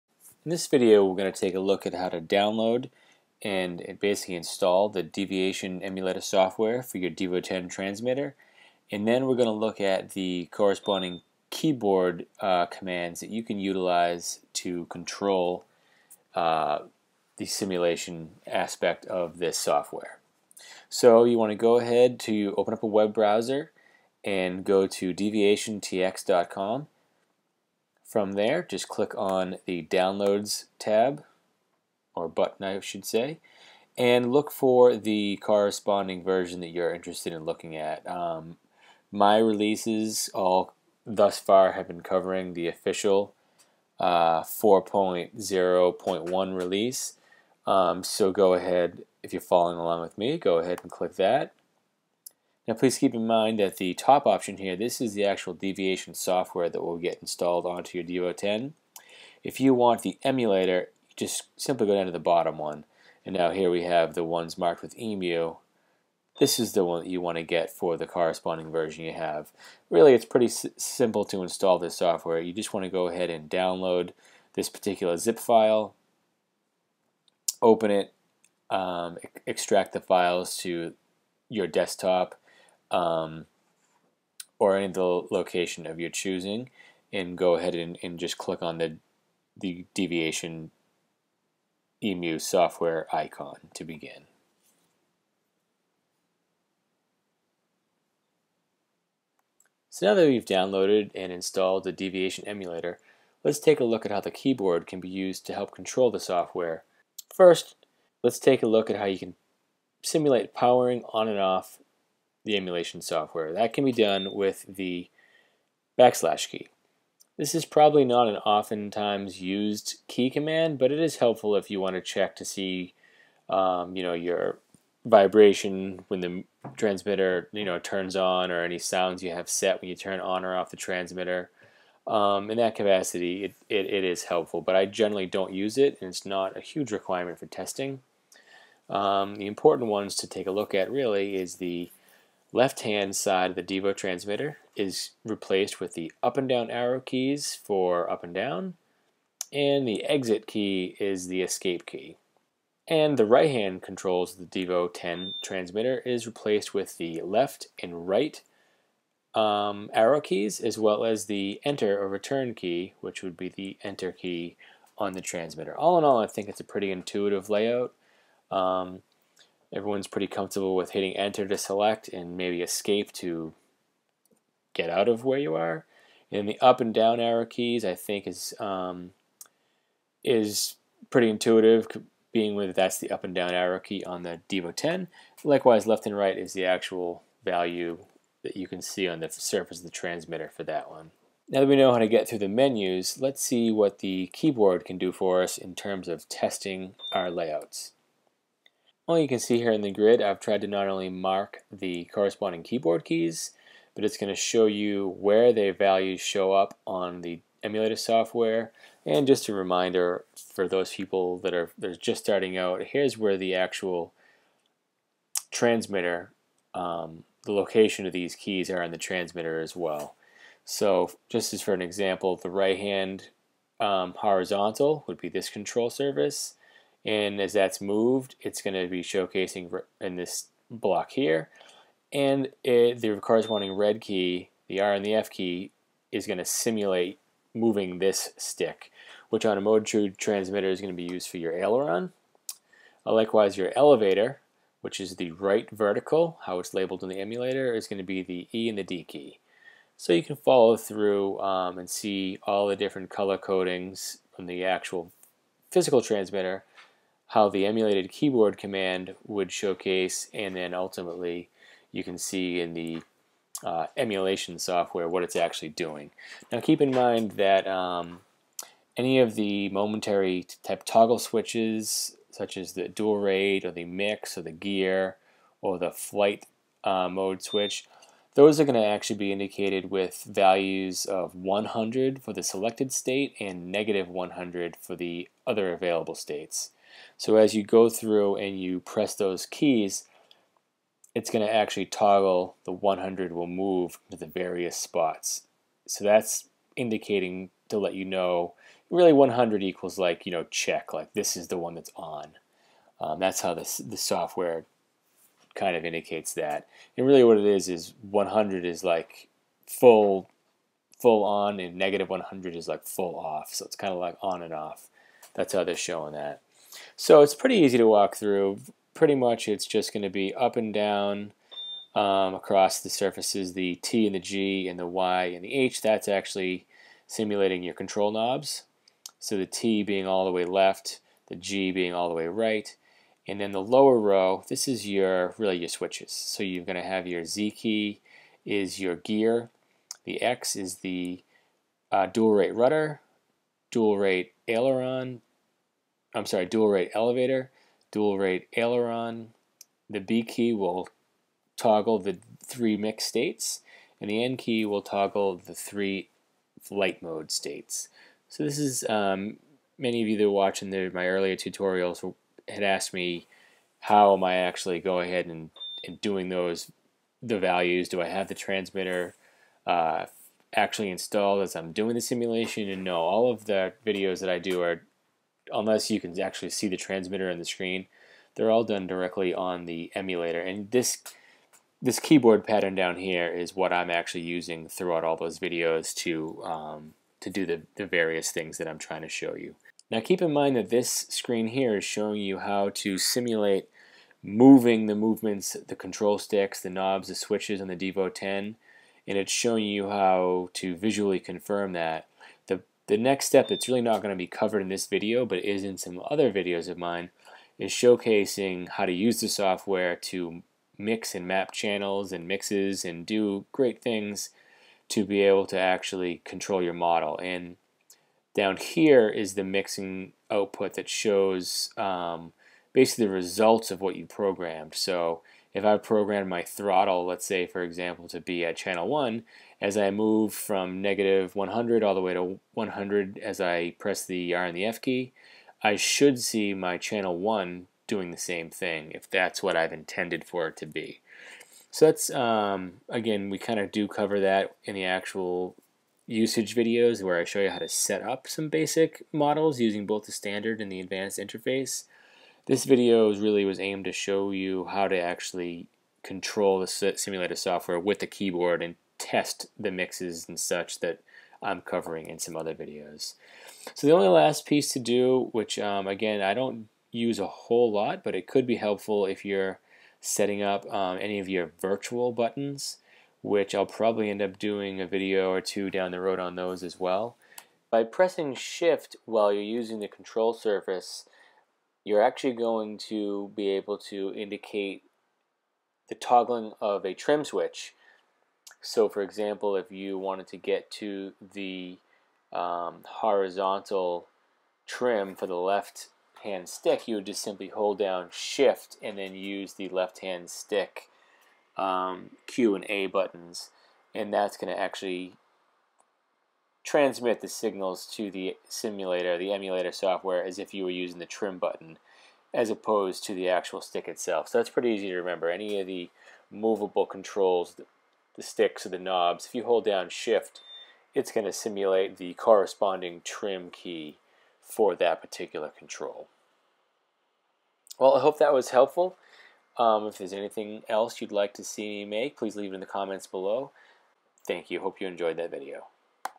Deviation Emulator Keyboard Layout
The aim of this video is to show you where to find the Deviation TX emulator software and show you how to use a computer keyboard to mimic the Transmitter controls to control the software. I had recieved alot of emails asking me how I was controlling the emulator software in my other videos so I wanted to make this video to help show what Im doing.